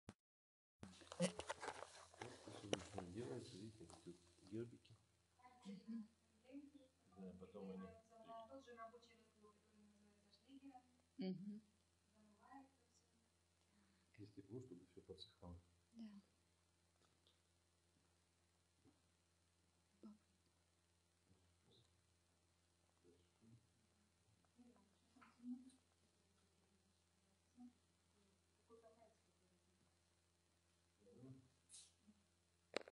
Fanagoria winery, Russia 22 10 2009
Here are some examples of ceramic production at Fanagoria winery. All is handmade.